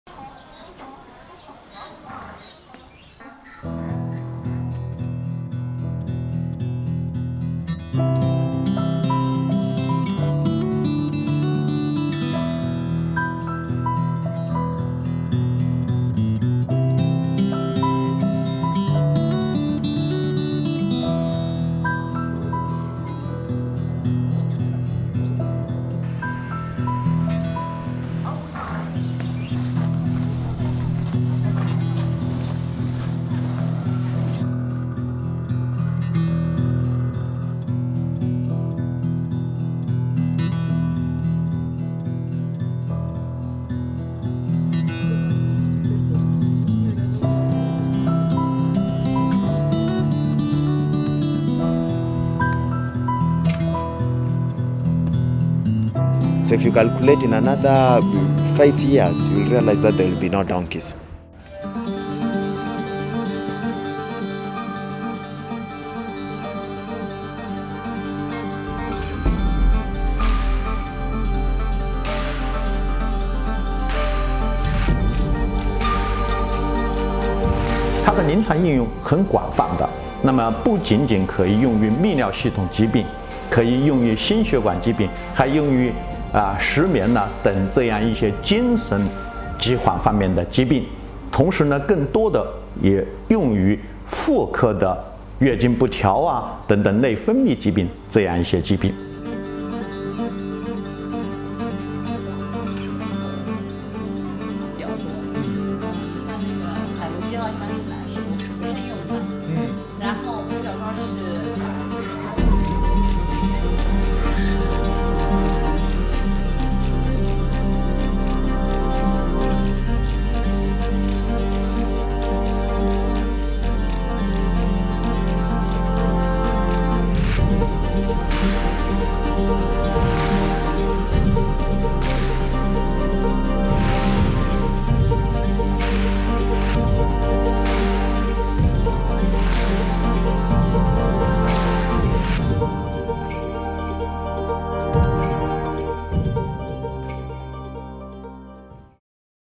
Хитой дориси учун дунë бўйлаб эшаклар қирғин қилинмоқда
Дастлаб чоб этилган